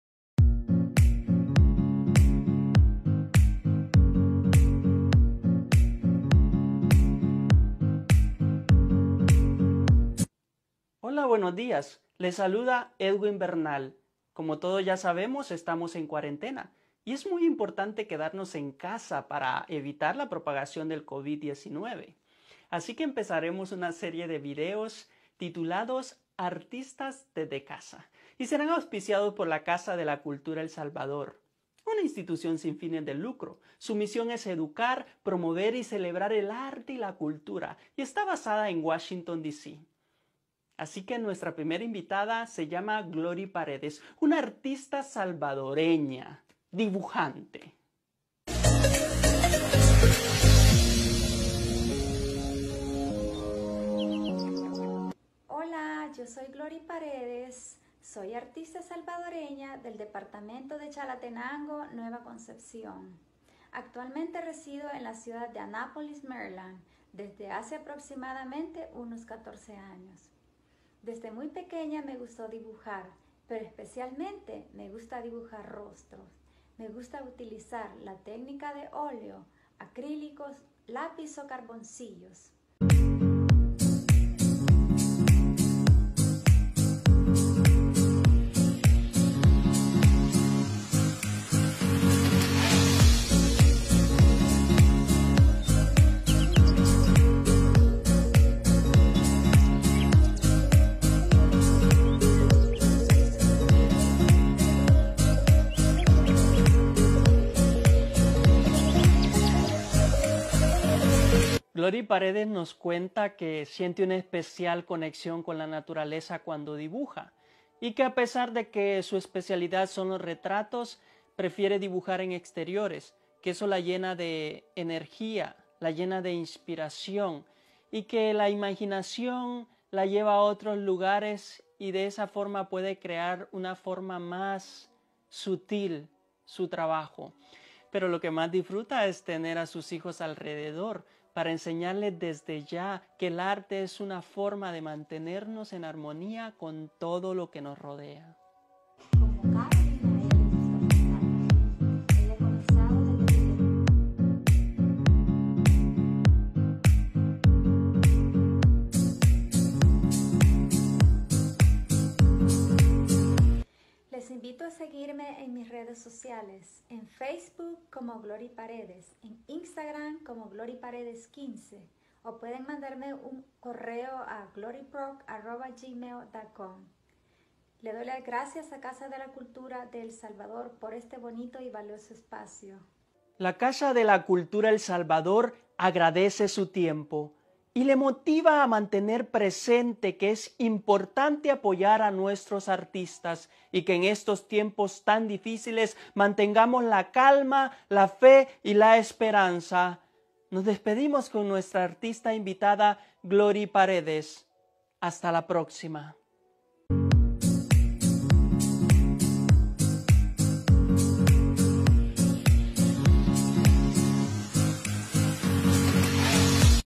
ARTISTAS DESDE CASA. Cápsula 1.Glory Paredes
Un esfuerzo de Casa de la Cultura El Salvador y el gobierno del Estado de Columbia para dar a conocer a los artistas salvadoreños y al mismo tiempos hacer conciencia sobre la importancia del distanciamiento social en tiempos del Covid 19